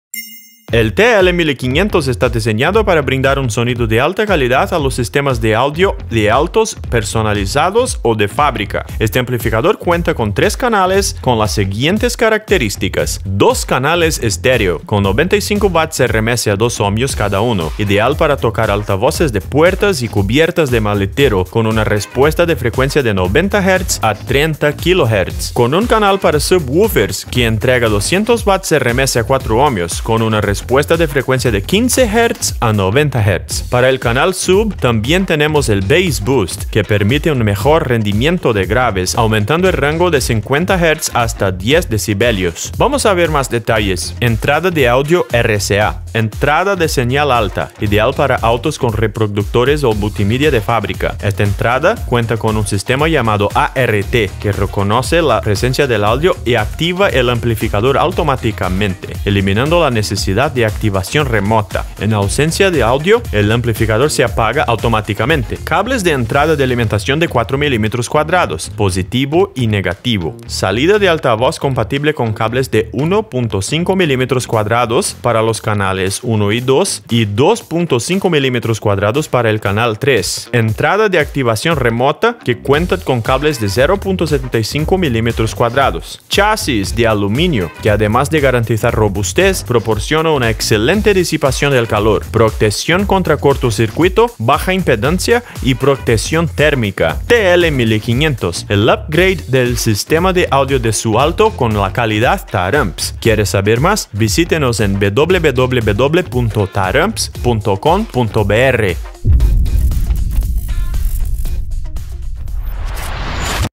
Amplificador Automotriz Taramps TL 1500 - 3 Canales - 390 Watts RMS
El amplificador TL 1500 de Taramps está diseñado para sistemas de audio para automóviles de 3 canales, y sus características son las siguientes:

- 2 canales estéreo (L / R) a 95 Watts RMS, a 2 ohms cada uno, con un filtro activo HPF de corte a 90Hz para conectar los altavoces de puerta y la tapa del maletero.
- 1 canal SUB con 200 vatios RMS a 4 ohmios, para conectar a altavoces de subgraves (Bajos). Este canal es el resultado de la suma de los canales (L / R) con filtro de corte activo LPF a 90Hz.
Es un amplificador estéreo, con una potencia total de 390 Watts RMS, FULL RANGO, que reproduce todas las frecuencias como más te guste.